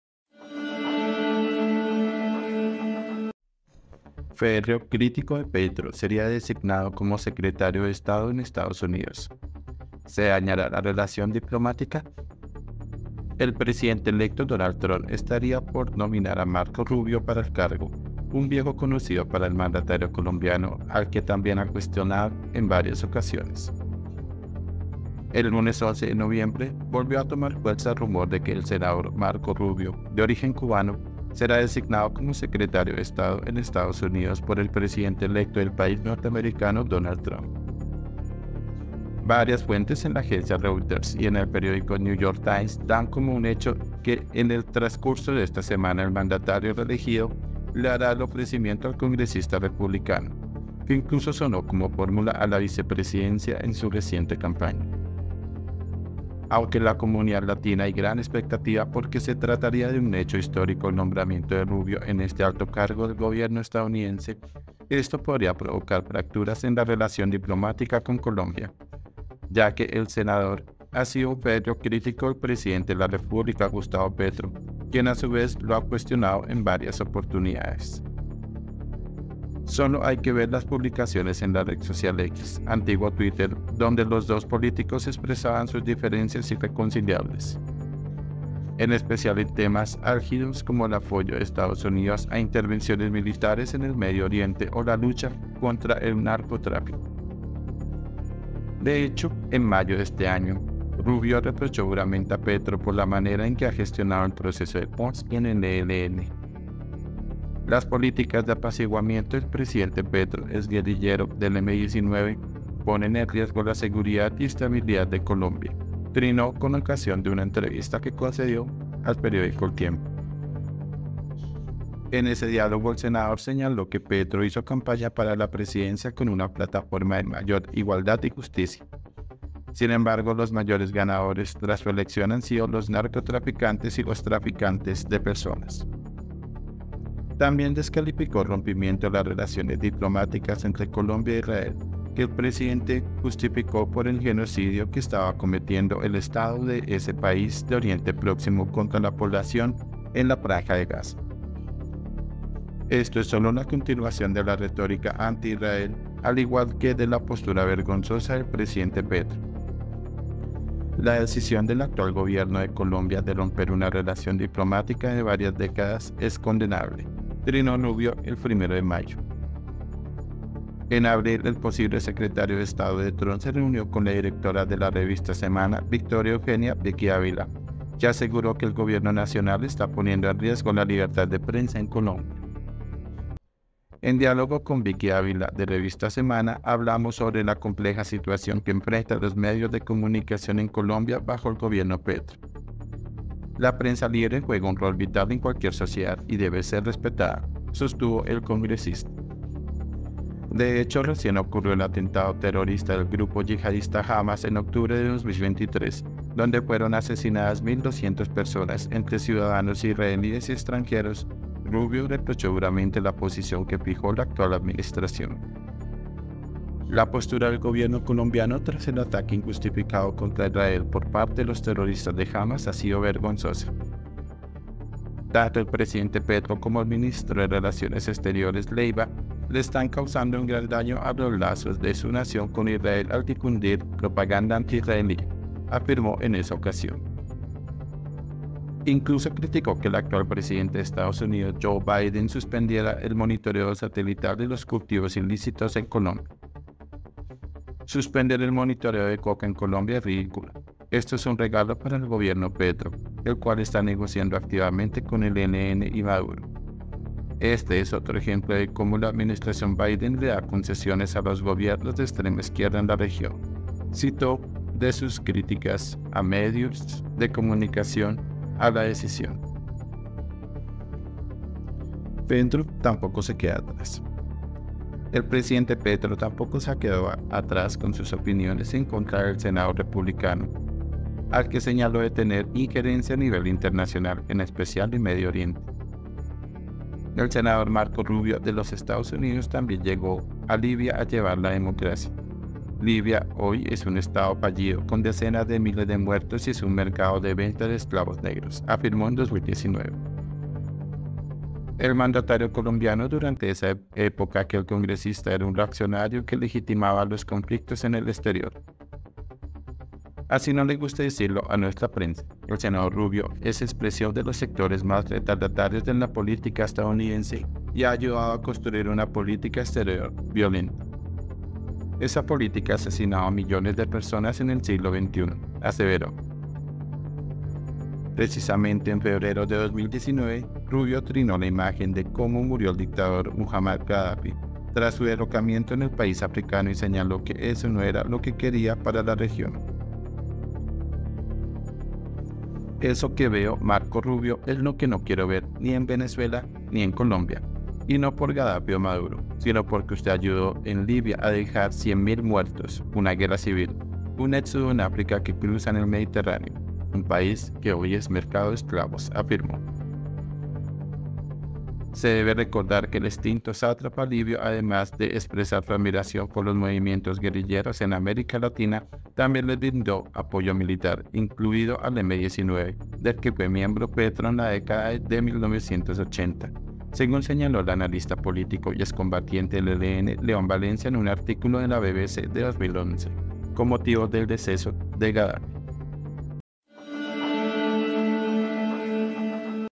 MALAS NOTICIAS PARA EL PRESIDENTE 🔴 Férreo crítico de Petro designado como secretario de Estado
Férreo crítico de Petro sería designado como secretario de Estado en EE.UU. ¿Se dañará la relación diplomática?
El presidente electo Donald Trump estaría por nominar a Marco Rubio para el cargo, un viejo conocido para el mandatario colombiano, al que también ha cuestionado en varias ocasiones
INFOBAE